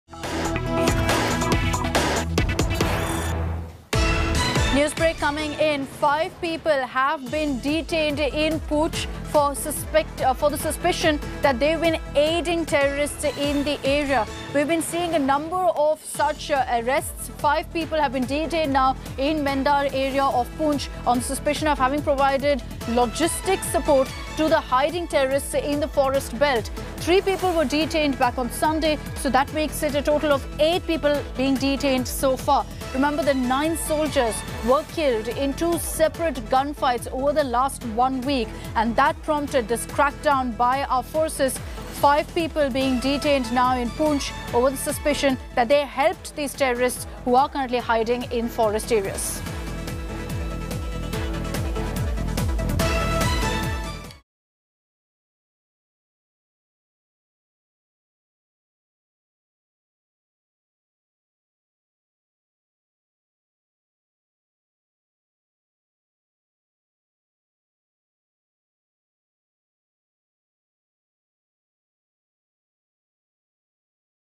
Poonch Encounter: Jammu-Kashmir Police Detains Five For Allegedly Providing Shelter To Terrorists
Entering day nine of the Poonch encounter, the Jammu and Kashmir police have now detained five persons for allegedly providing shelter to the terrorists. Earlier, the police had detained three people for allegedly providing logistical support to the infiltrators. The Indian Army on Monday continued the anti-terror operations at the Poonch encounter site during the night. This was the first time, the Army carried out the operation after dark.